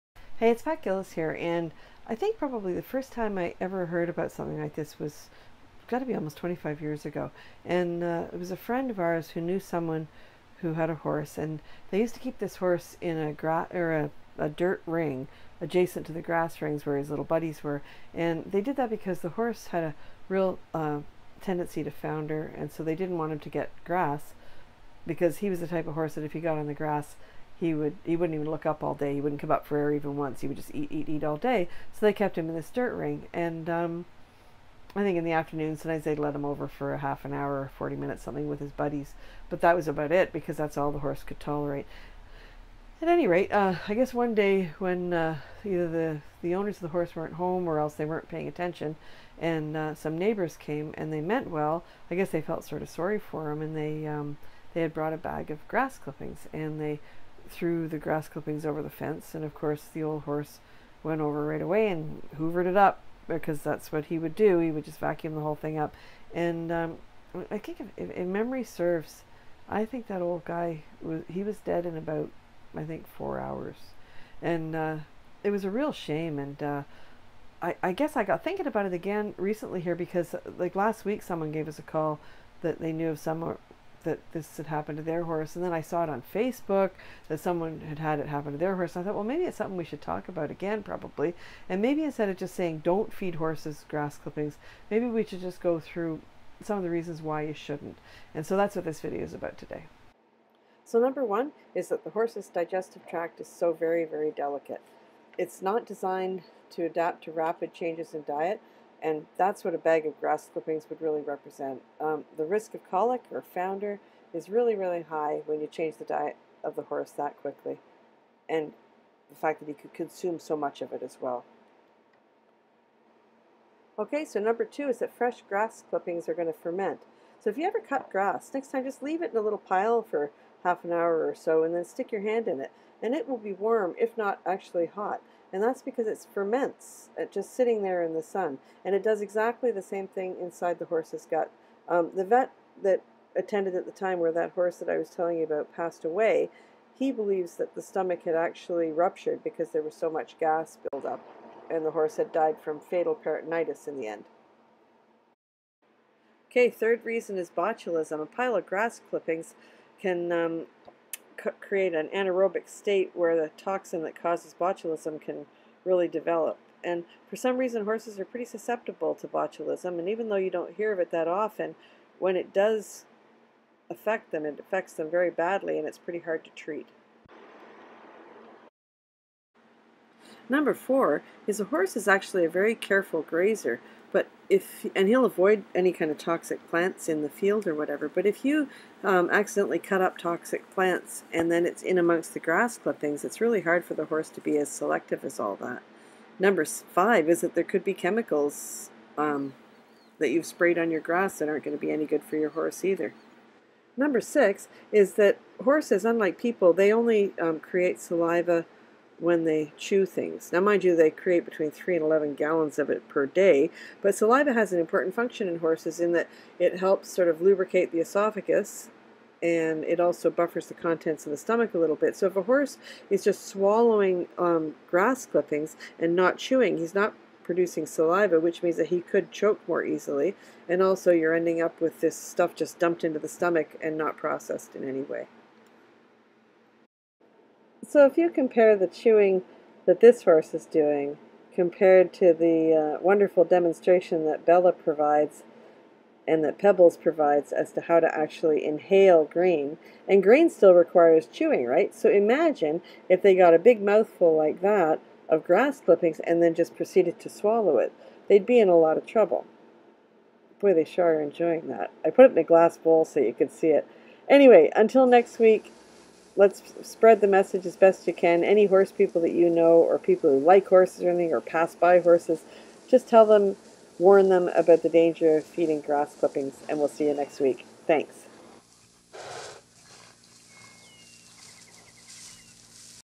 Just say NO to grass clippings.
Feeding horses grass clippings from your lawn might seem like a good idea but it's really not worth the risk.